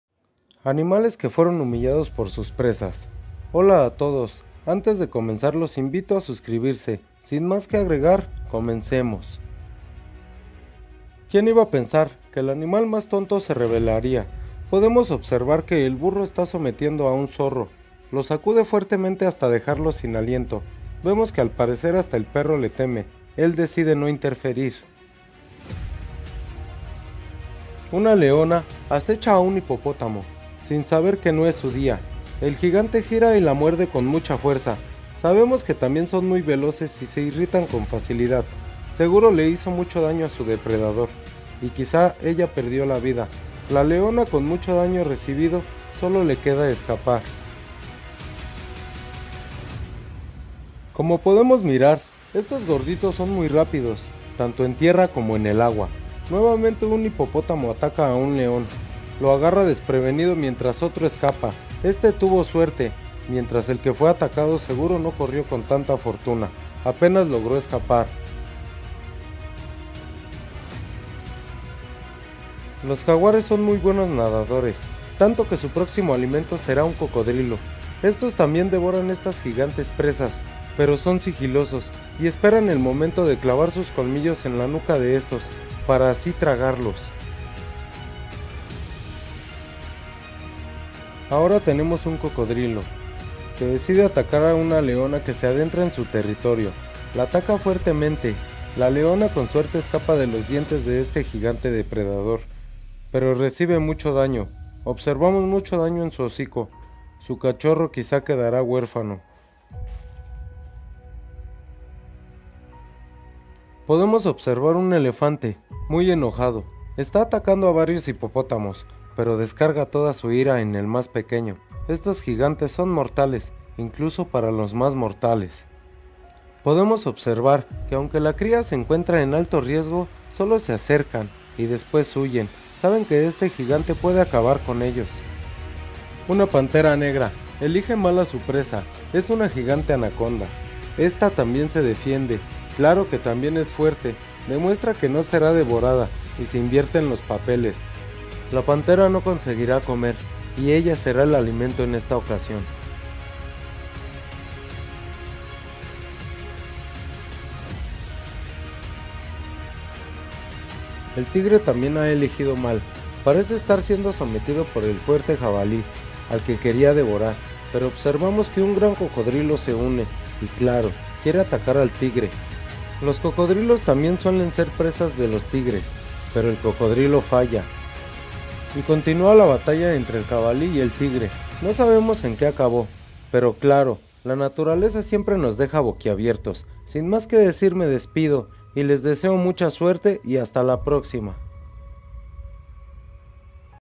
Depredadores humillados por sus presas
Les traigo un video en el que grandes y fuertes depredadores son humillados por presas espero les guste el contenido sigo poniendo esfuerzo para poder mejorar el contenido espero les agrade el contenido.

atribuyo el contenido a los dueños de las imagenes gracias.